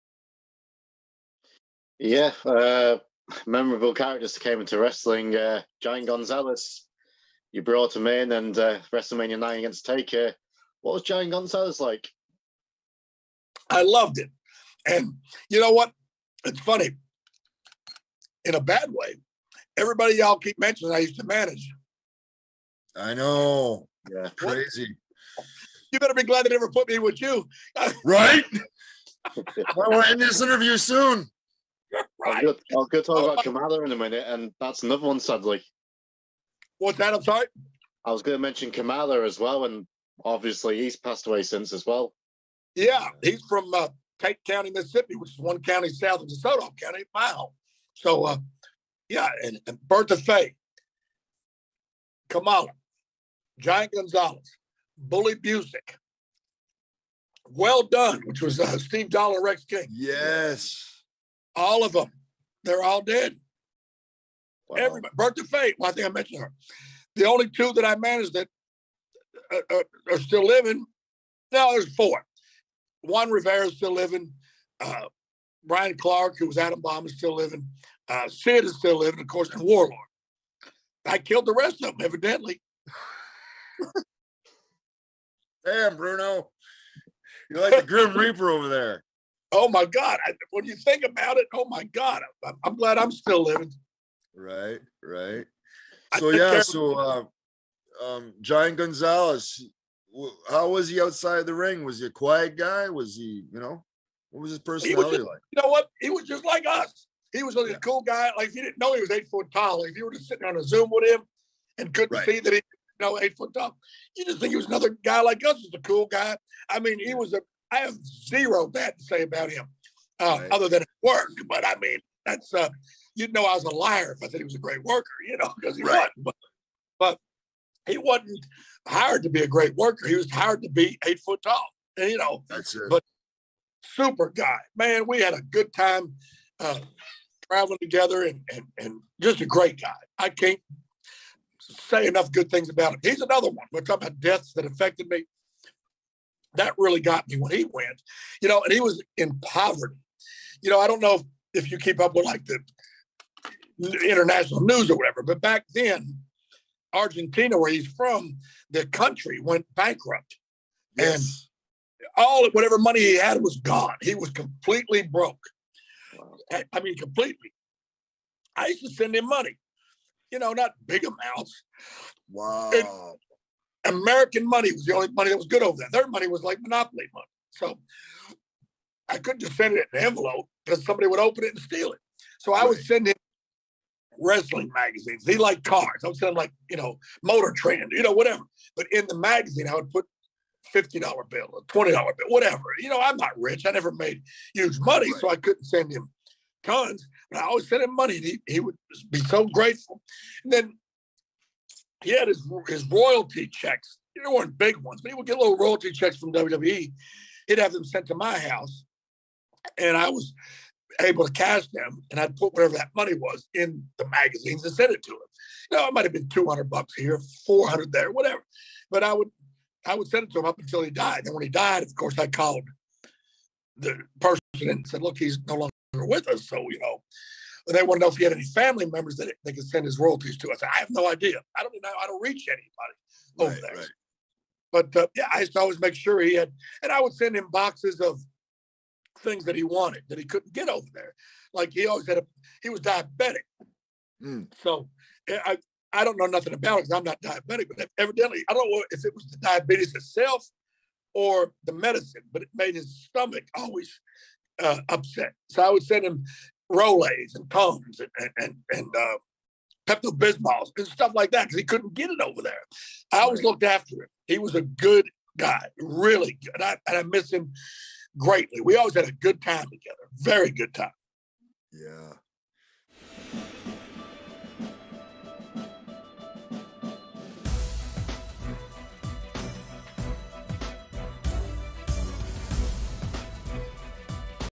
Harvey wippleman Remembers Giant Gonzales
harvey wippleman talks managing giant gonzales and how he helped him